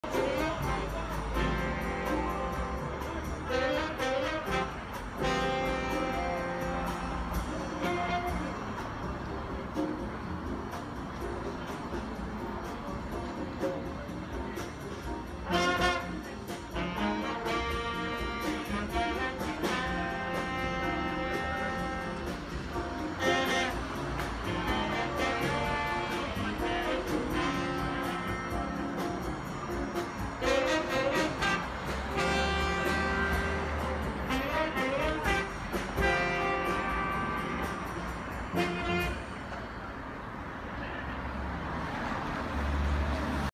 I love Hitchin at master car sales listening to jazz music from the Victoria pub
Master car sales Hitchin